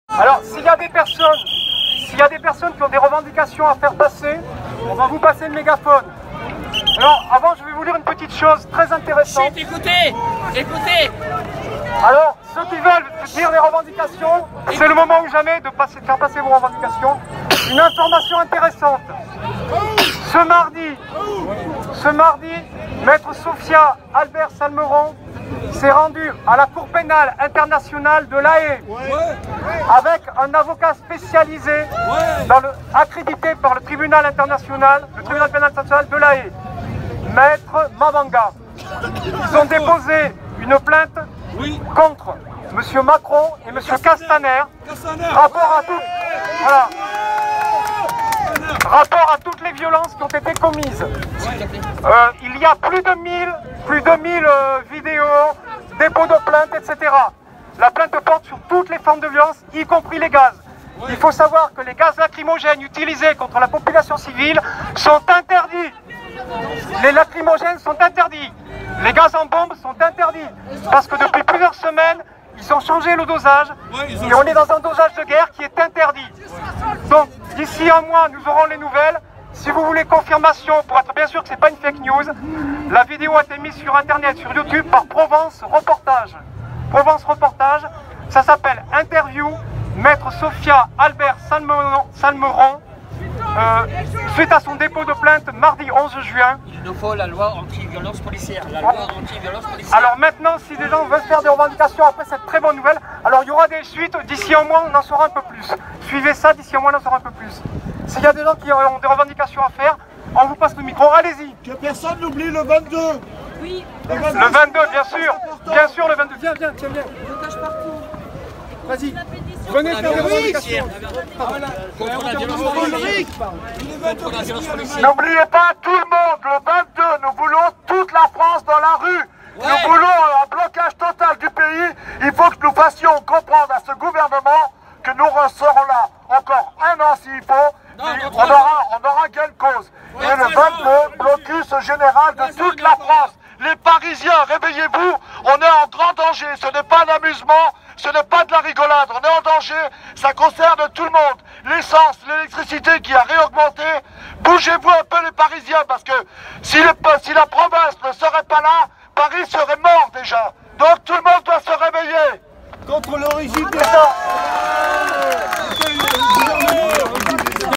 «Les gaz lacrymo utilisés contre la population sont interdits..On est dans un dosage de guerre »
13:38 «Ce mardi, maitre Sophia Albert Salmeron s’est rendu à la cour pénale internationale de la Haye avec un avocat spécialisé, accrédité par le tribunal international de la Haye Maître Mavinga. Ils ont déposé une plainte contre Monsieur Macron et Monsieur Castaner en rapport avec toutes les violences qui ont été commises. Il y a plus de 1000 vidéos, dépôts de plaintes, etc. La plainte porte sur toute sortes de violences y compris les gaz. Il faut savoir que les gaz lacrymogènes utilisés contre la population civile sont interdits..depuis plusieurs semaines ils ont changés le dosage. On est dans un dosage de guerre qui est interdit. D’ici un mois nous aurons des nouvelles... »